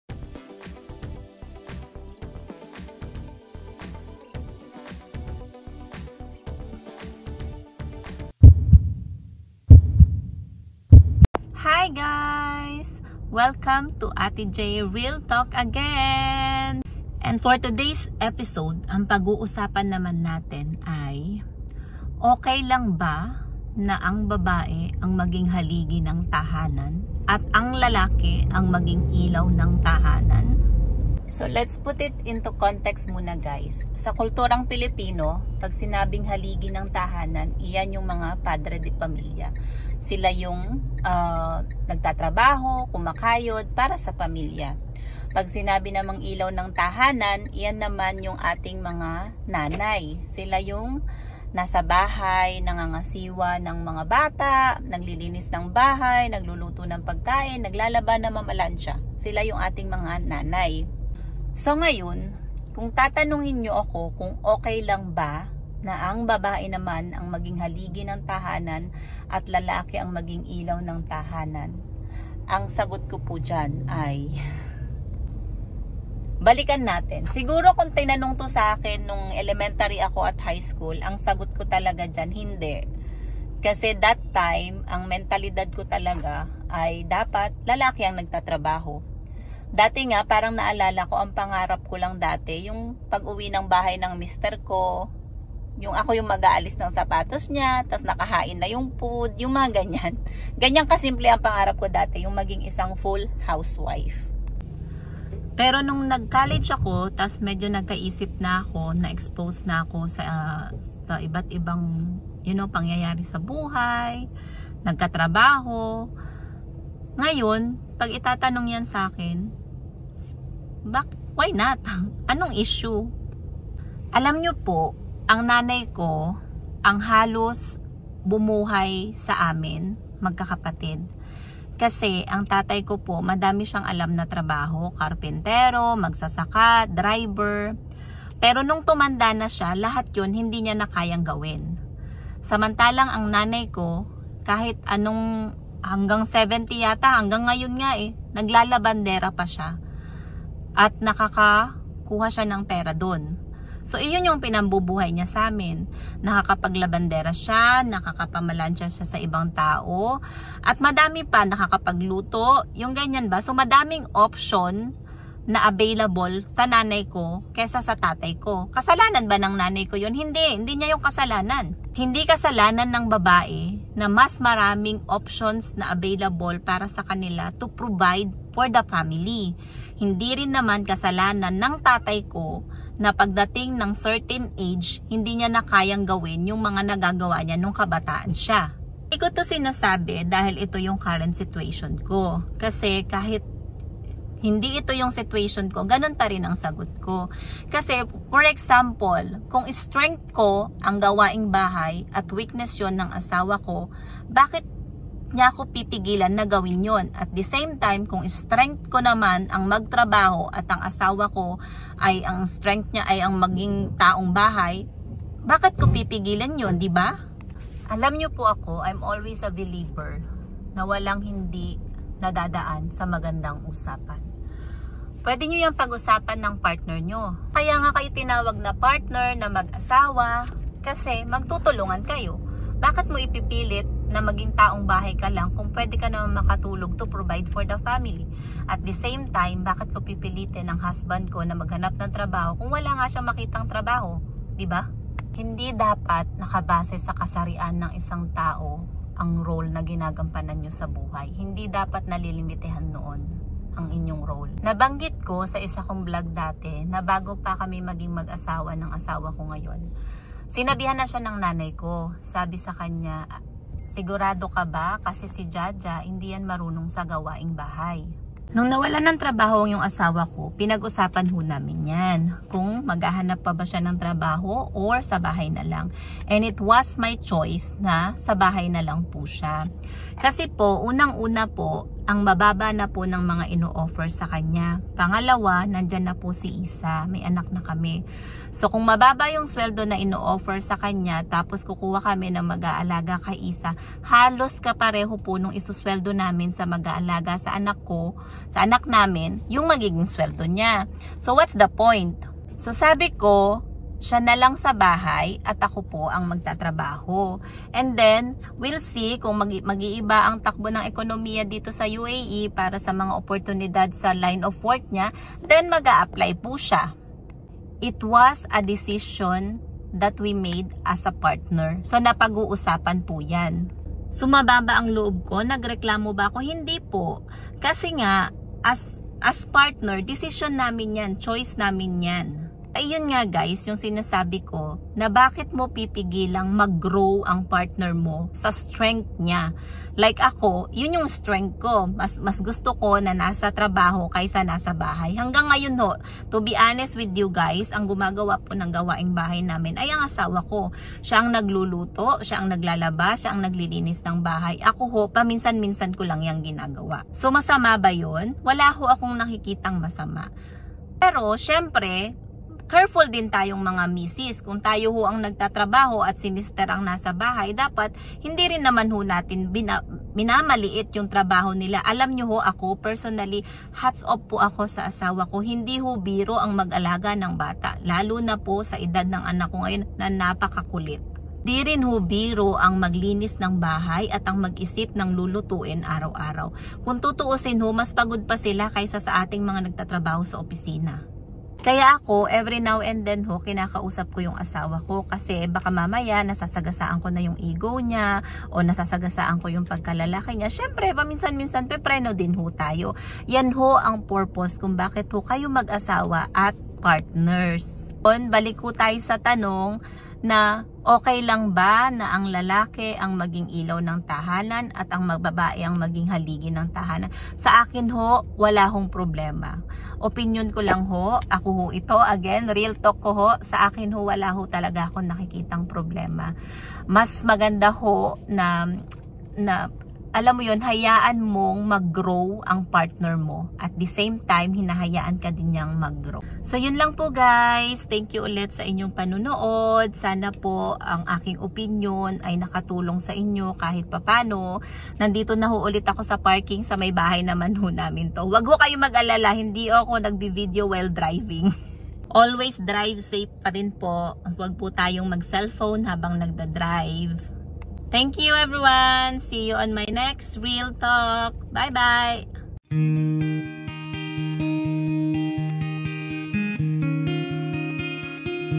OK LANG BA? palit tayo - ikaw ang ilaw at ako ang haligi ng tahanan💪
Real talk #2: Ok lang ba na ang babae ang haligi ng tahanan at lalaki ang ilaw nito? Ano sa palagay niyo?